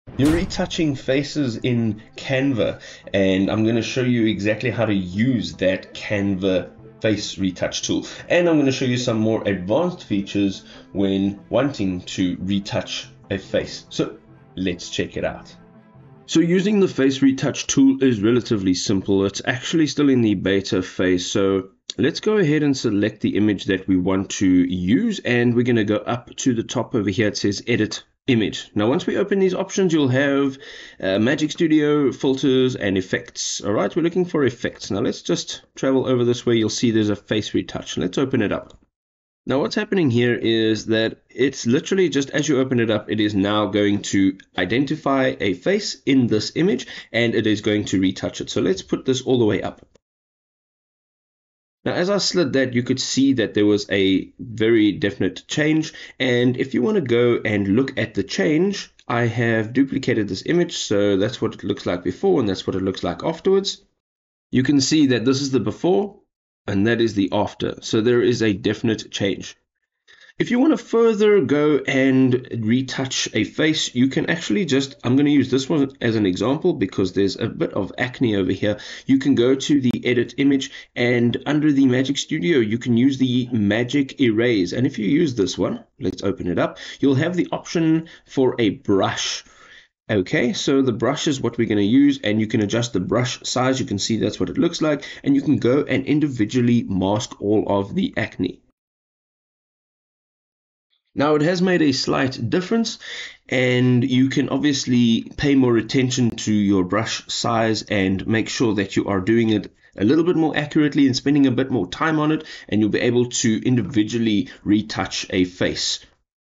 How to Use Face Retouch in Canva | Canva Tutorial 2025
In this video, "How to Use Face Retouch in Canva," we’ll show you how to enhance your portraits and selfies with Canva’s powerful face retouching tools. Whether you want to smooth skin, brighten eyes, or remove blemishes, Canva makes it easy to achieve a polished look for your images. We’ll guide you through the step-by-step process of accessing the face retouch features, including Skin Smooth, Blemish Remover, and Eye Brightening. You’ll learn how to adjust these settings to achieve natural-looking results while maintaining the integrity of your photos. This tutorial is perfect for social media managers, bloggers, and anyone looking to improve their personal or professional images. Join us and discover how to elevate your portrait editing skills using Canva's face retouch tools!

Timestamps

Intro – 00:00 – 00:15
Using Face Retouch in Canva – 00:16 – 01:56